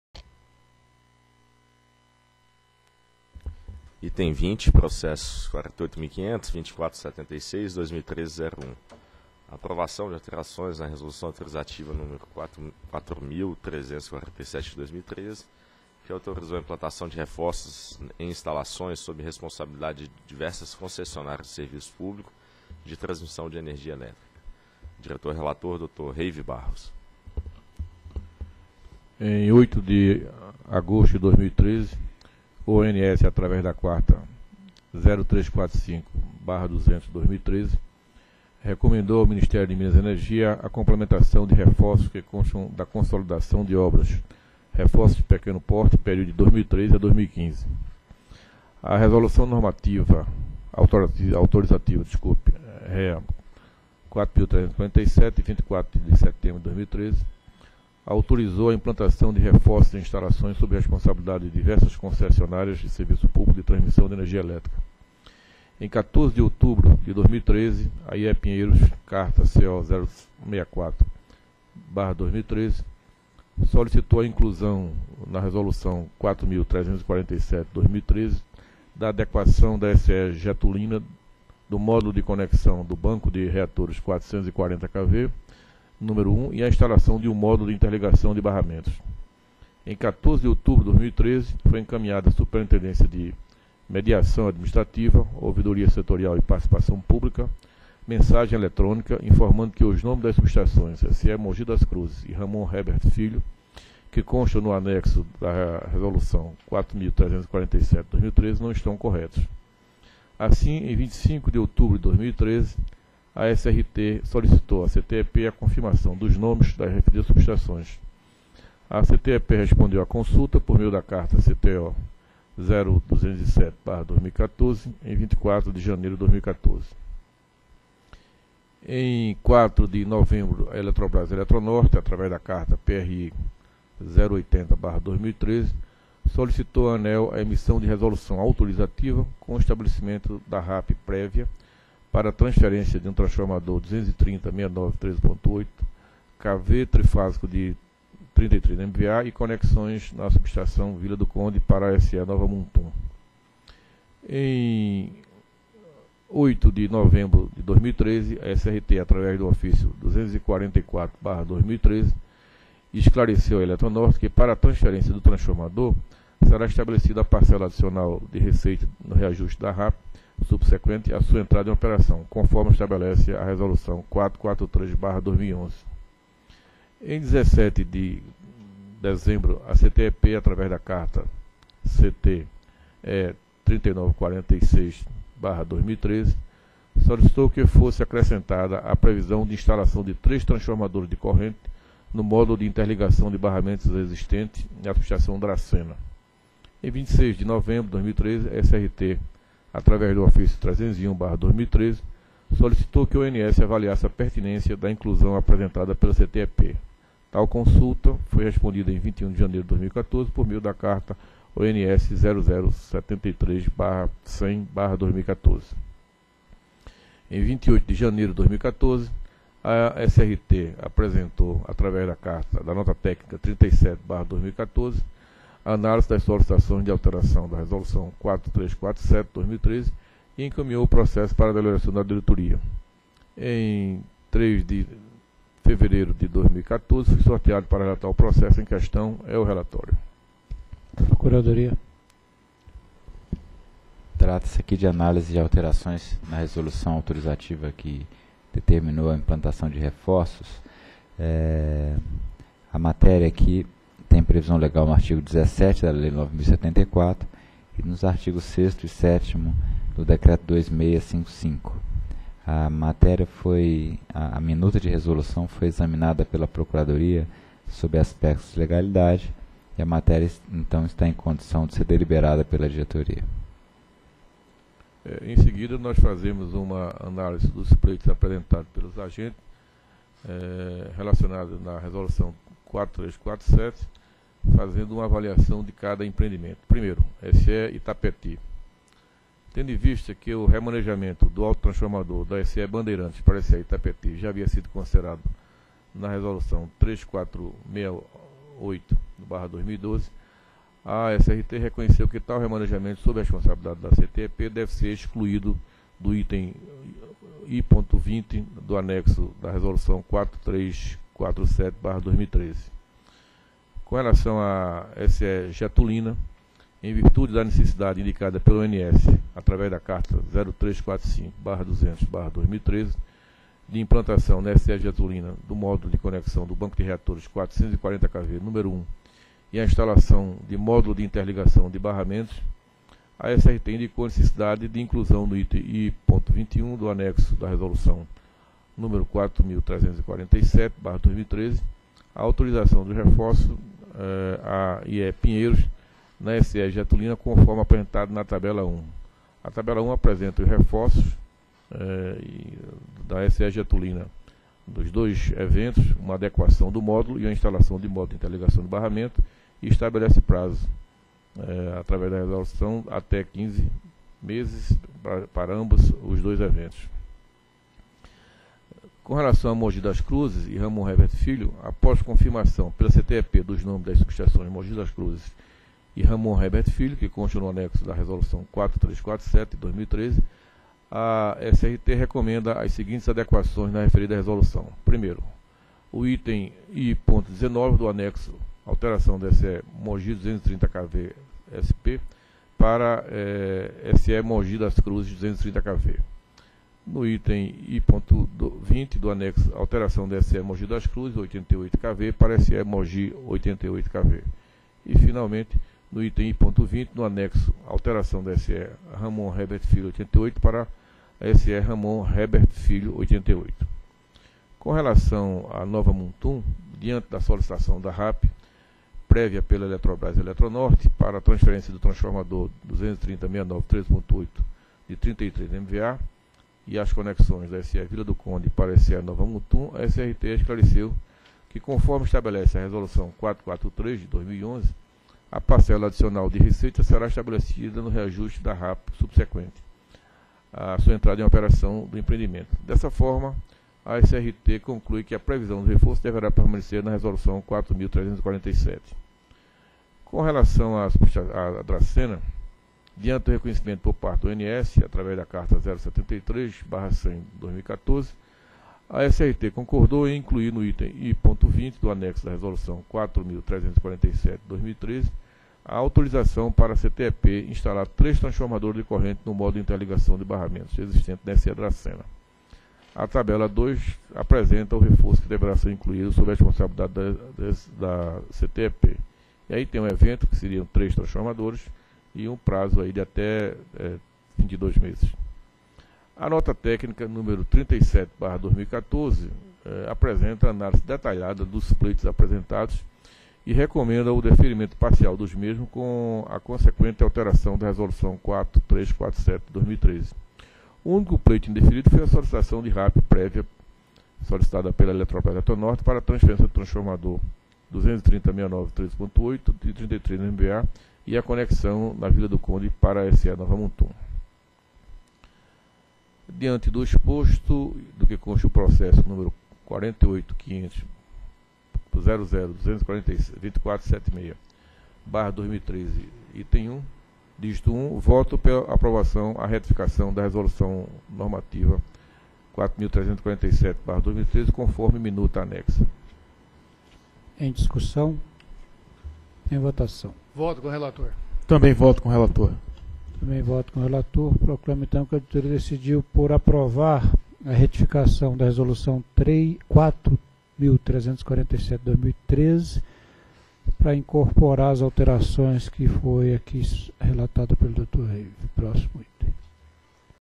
Item 20 - 4ª Reunião Pública Ordinária ANEEL 11/02/2014
Assunto: Aprovação de alterações na Resolução Autorizativa nº 4.347/2013, que autorizou a implantação de reforços em instalações sob responsabilidade de diversas concessionárias de serviço público de transmissão de energia elétrica. Área Responsável: Diretoria -- DIR.